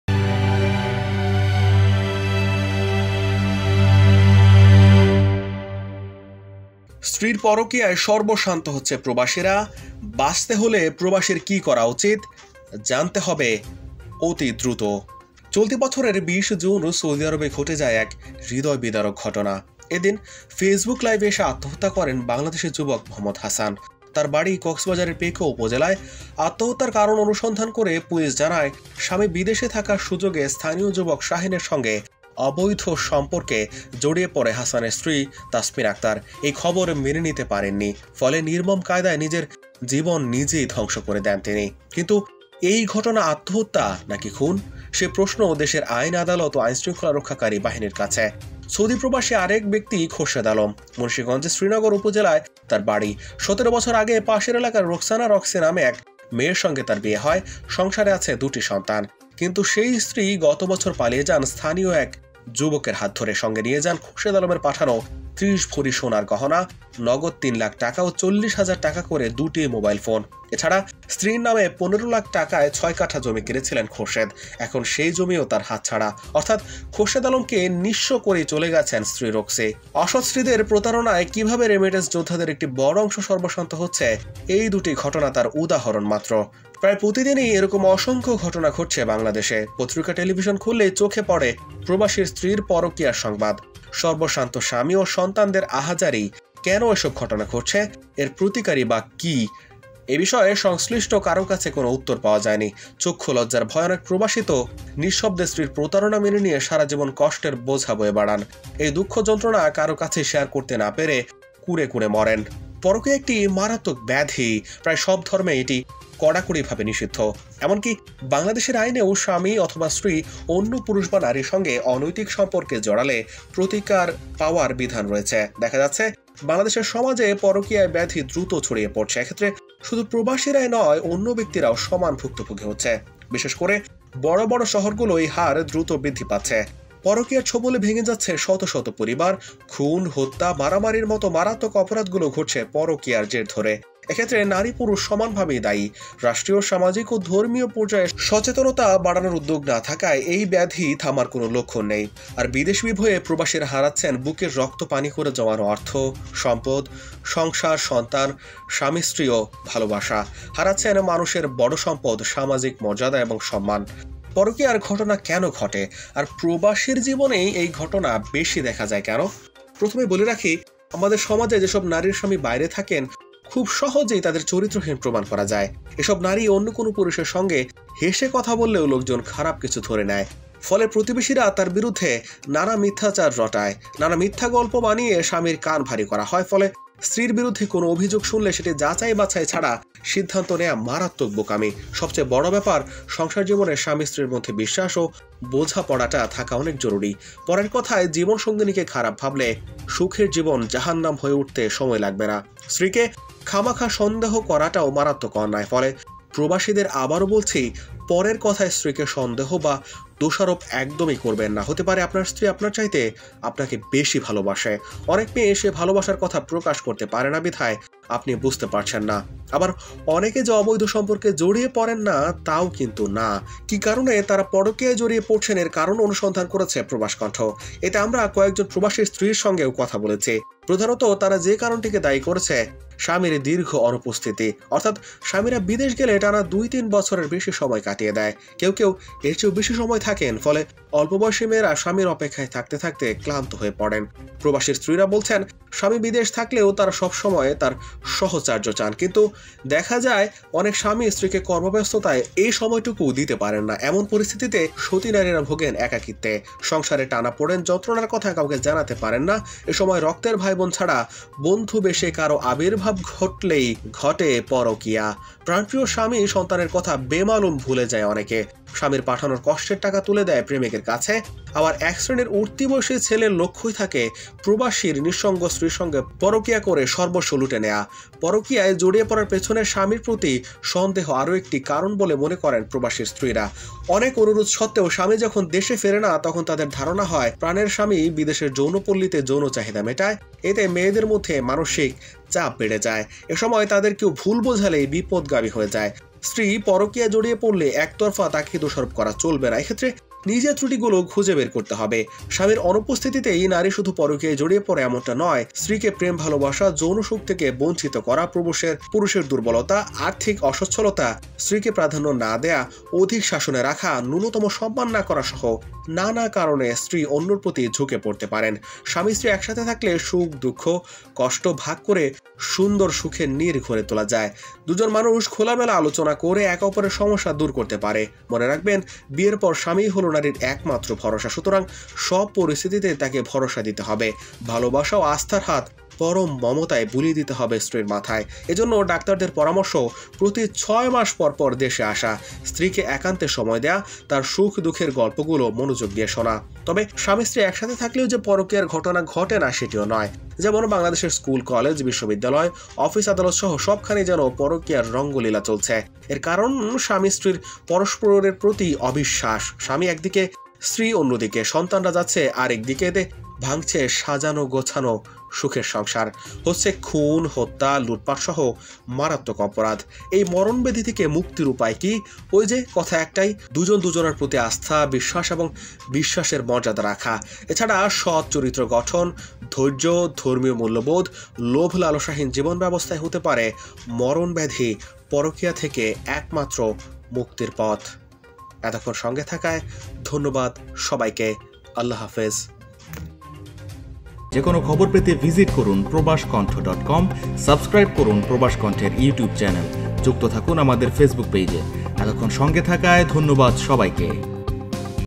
স্ত্রীর পরকীয়ায় সব হারিয়ে পথের ফকির প্রবাসীরা। জানুন কিভাবে রোধ করবেন।
সম্পাদক
প্রবাস কন্ঠ